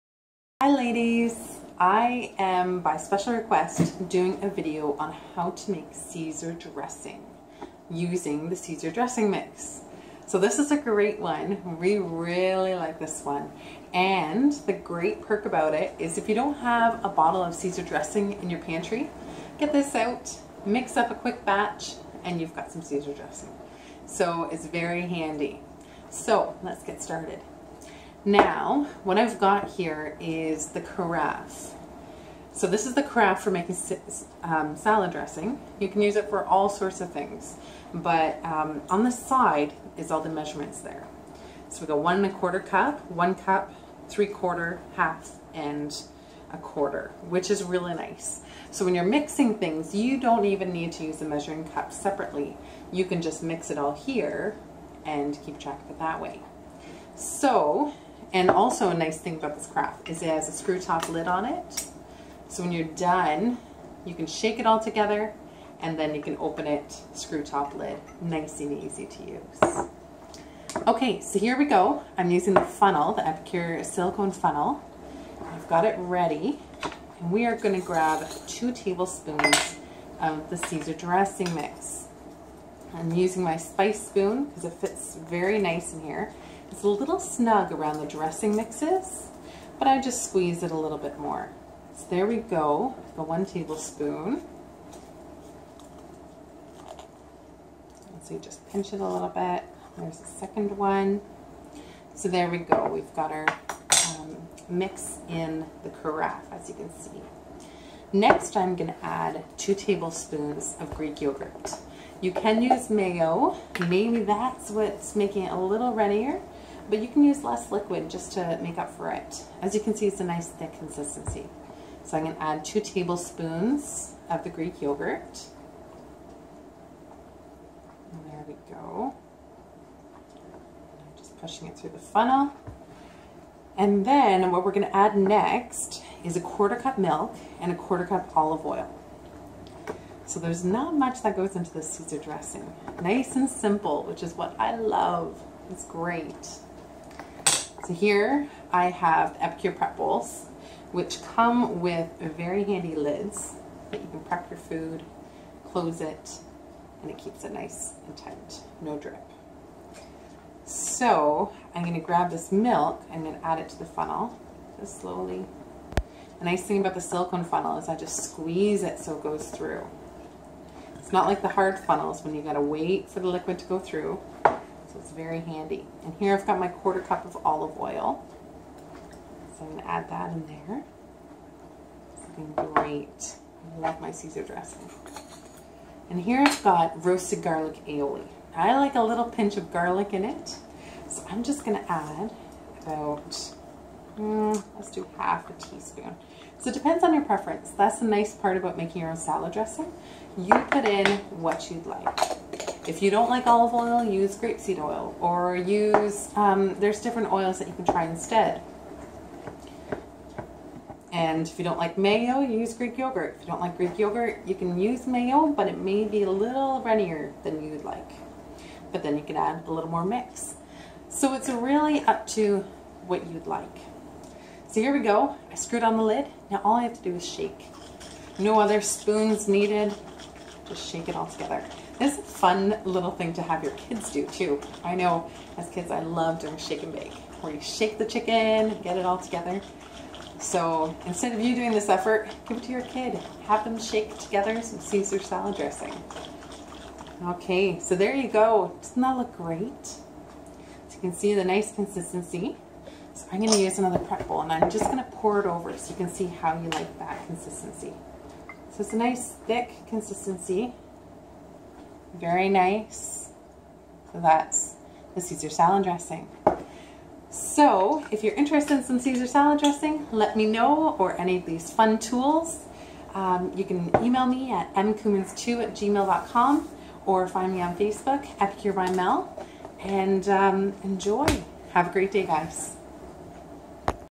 Making Epicure Caesar Salad Dressing
Epicure salad dressing is so easy to make and you can customize it how you like it! Run out of Caesar dressing in your house a lot? Have the mix handy and you can mix up a fresh batch in seconds!